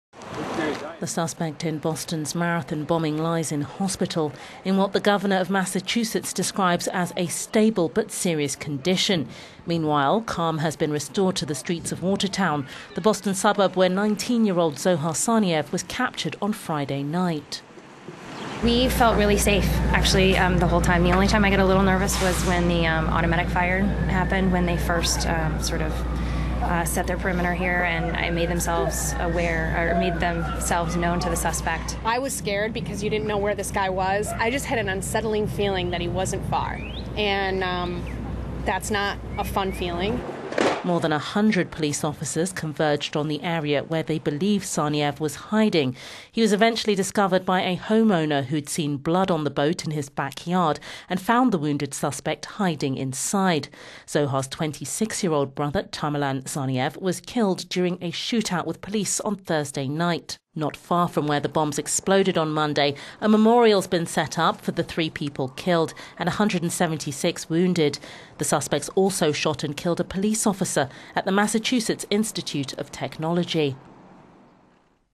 Boston bombing suspect remains in hospital
The suspect in Boston's marathon bombing lies in hospital in what the Governor of Massachusetts Deval Patrick describes as a "stable, but serious condition".

Dzhokhar Tsarnaev was shot in the throat and has not yet been able to speak, according to a source close to the investigation

Meanwhile, calm has been restored to the streets of Watertown - the Boston suburb where Tsarnaev, 19, was captured on Friday night.

Joy Arcolano, who lives close to where Tsarnaev was caught, said: "We felt really safe, actually, the whole time. The only time I got a little nervous was when the automatic fire happened, when they first, sort of, set their perimeter here and made themselves known to the suspect."

Another neighbour Leah Flynn added: "I was scared because you didn't know where this guy was. I just had an unsettling feeling that he wasn't far, and that's not a fun feeling."

More than 100 hundred police officers converged on the area where they believed Tsarnaev was hiding on Friday night.

Tsarnaev was eventually discovered by a homeowner who had seen blood on the boat in his backyard and found the wounded suspect hiding inside.

Tsarnaev's brother, Tamerlan Tsarnaev, 26, was killed during a shootout with police on Thursday night.

Not far from where the two bombs exploded on Monday, a memorial has been set up for the three people killed and 176 wounded.

The suspects also shot and killed a police officer at the Massachusetts Institute of Technology.